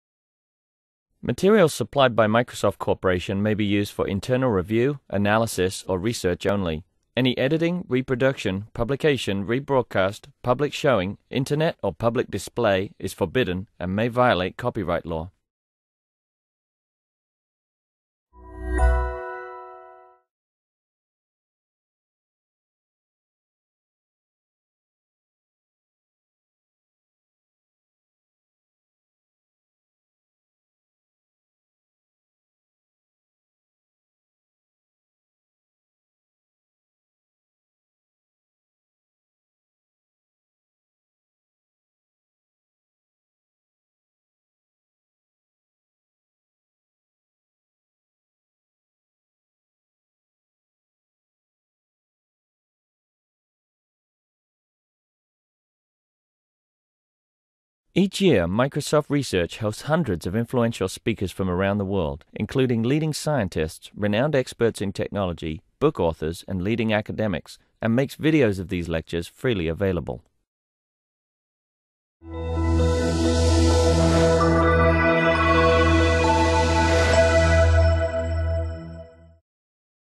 DNA Strand Displacement
A 3D animation of DNA strand displacement.